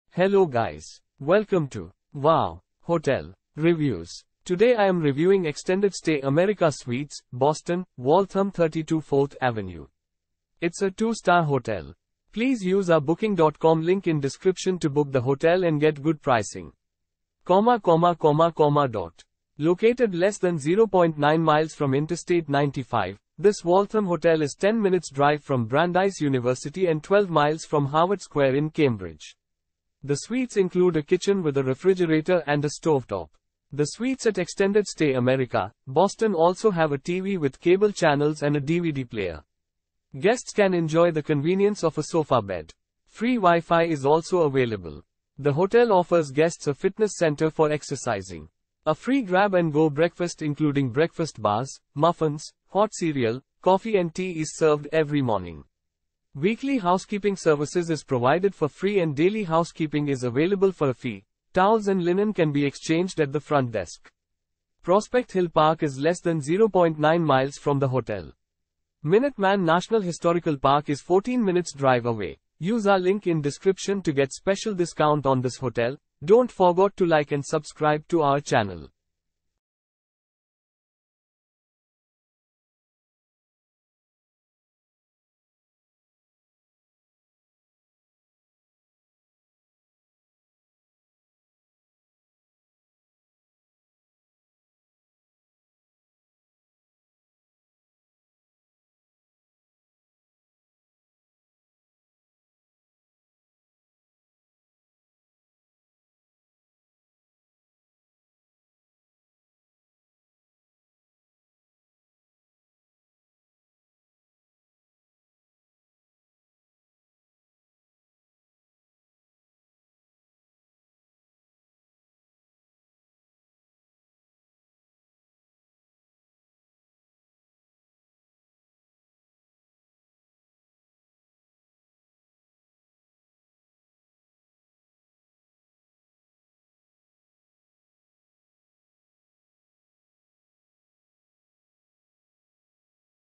Extended Stay America Suites - Boston - Waltham - 32 4th Ave Review - Waltham , United States of Ame
Extended Stay America Suites - Boston - Waltham - 32 4th Ave

Located less than 0.9 mi from Interstate 95, this Waltham hotel is 10 minutes’ drive from Brandeis University and 12 mi from Harvard Square in Cambridge. The suites include a kitchen with a refrigerator and a stovetop.

The suites at Extended Stay America – Boston also have a TV with cable channels and a DVD player. Guests can enjoy the convenience of a sofa bed. Free WiFi is also available.

The hotel offers guests a fitness center for exercising. A free grab-and-go breakfast including breakfast bars, muffins, hot cereal, coffee and tea is served every morning. Weekly housekeeping services is provided for free and daily housekeeping is available for a fee; towels and linen can be exchanged at the front desk.

Prospect Hill Park is less than 0.9 mi from the hotel. Minute Man National Historical Park is 14 minutes’ drive away.